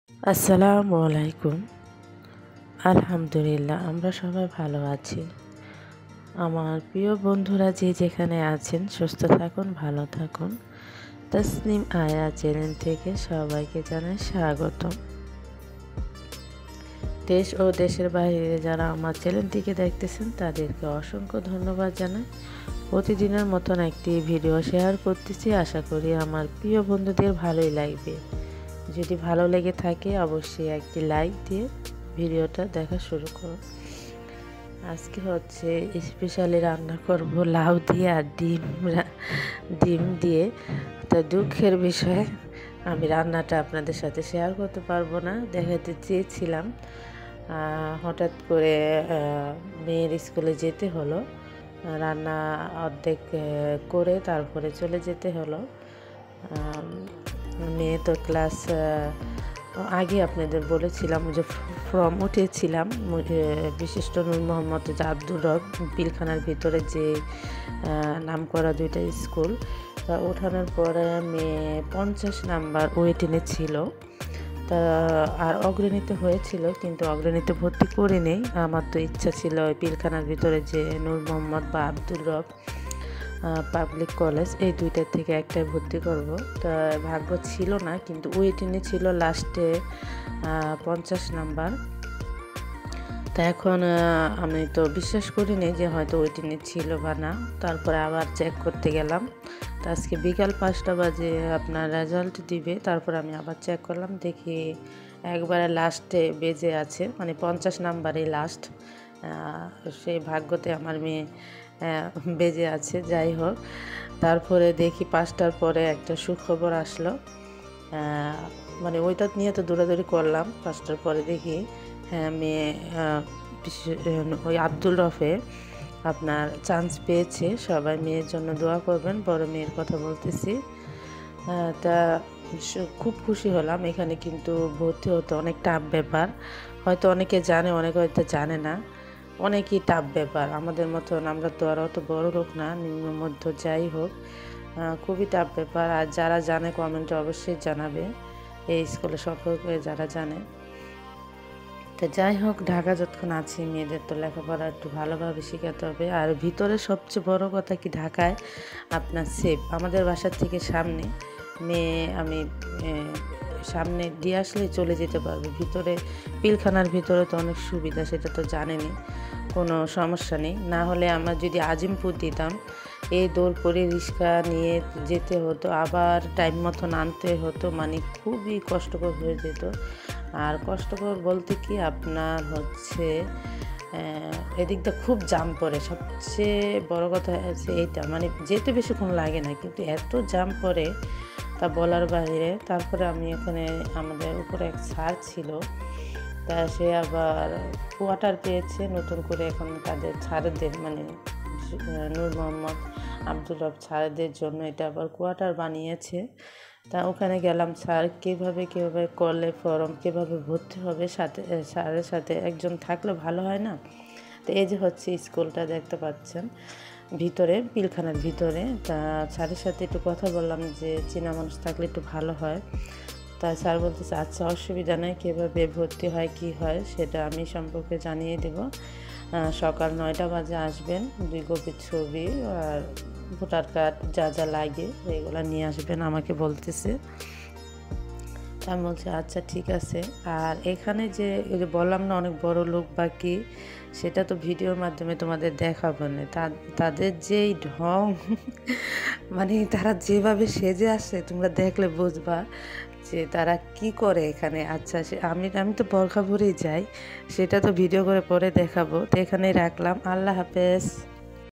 কি এমন খবর আসলো রান্না রেখেই চলে গেলাম#tasnimayra
আজকে আপনাদের সাথে শেয়ার করবো লাউ দিয়ে ডিম রান্না আর হচ্ছে একটা সুখবর জানাবো মেয়ে মুন্সি আব্দুর রউফ স্কুলের চান্স পেয়েছে সেটি আপনাদের সাথে শেয়ার